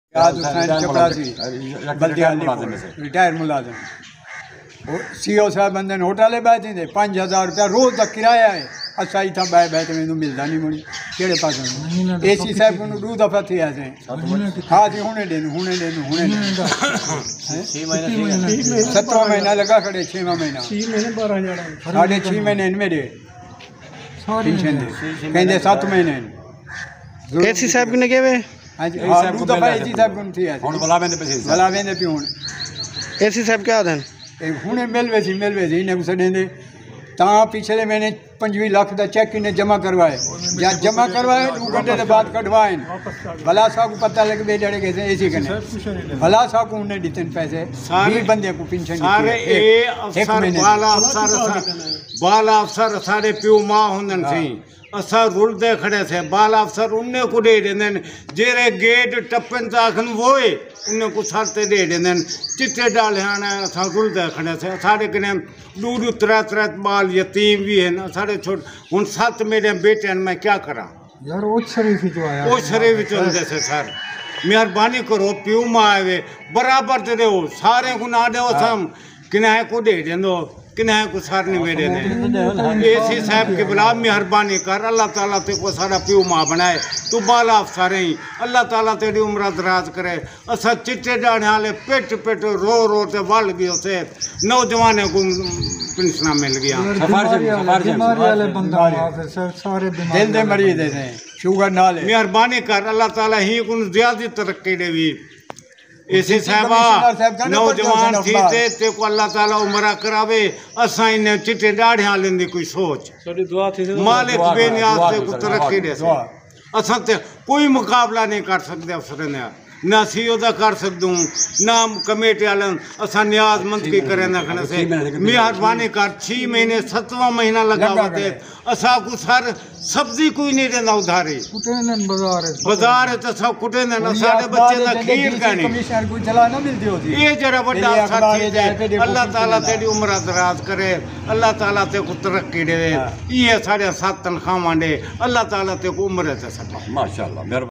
Retired Baldiya native in Alipur accused of not getting salary for 6 months | Punjnad Tv
Alipur retired municipal employees are not being given pension for 6 months. Municipality employees protest

Breaking News | Latest News | Alipur | Retired municipal employees | 6 months | Employees protest | DC Muzaffargarh | AC Alipur